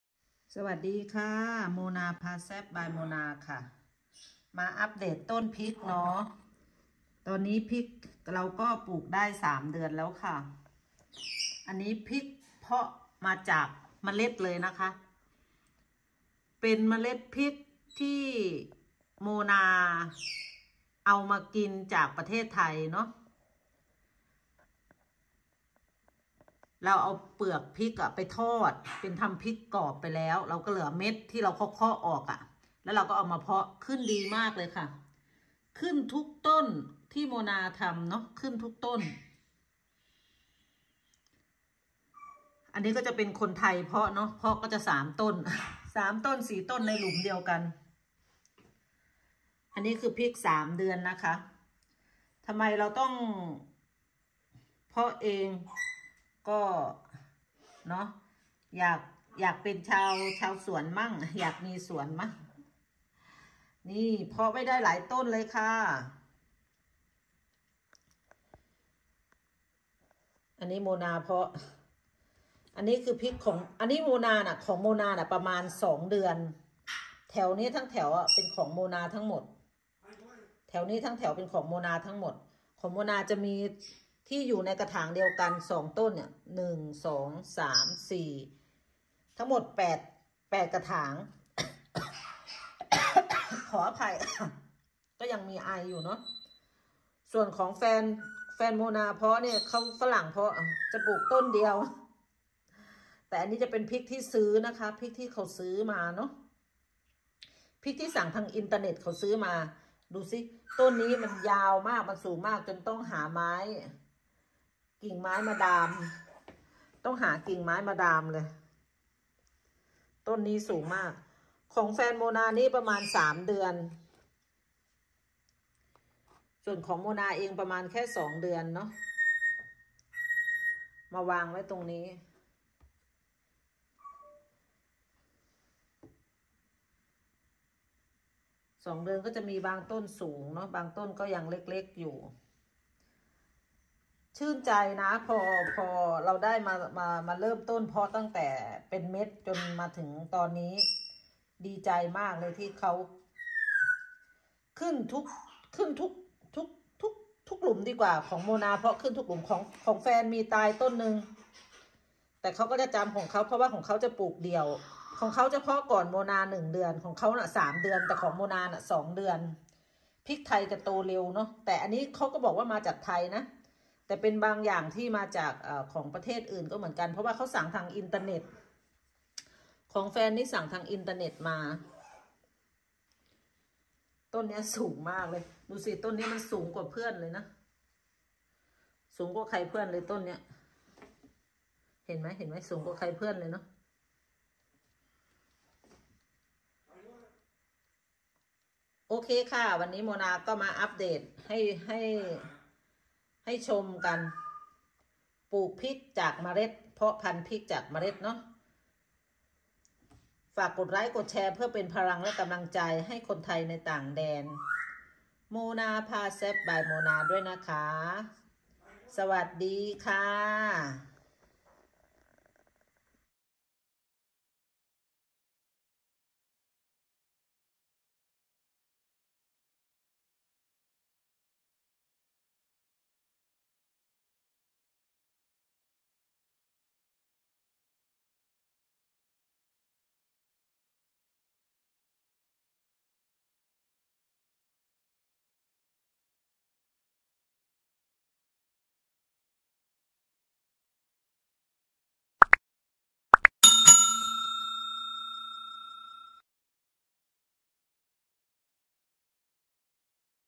อัพเดท เพาะเม็ดพริกจากไทย 2,3 เดือนโตขนาดใหนแล้ว
อัพเดทพริกที่เพาะจากเม็ดค่อยๆโตแล้วคะ ตื่นเต้นตลอดเพราะเราเพาะเองจากมือเราในบ้าน เพราะอยู่เมืองหนาวเย็นต้องเพาะต้นพริกในบ้านคะ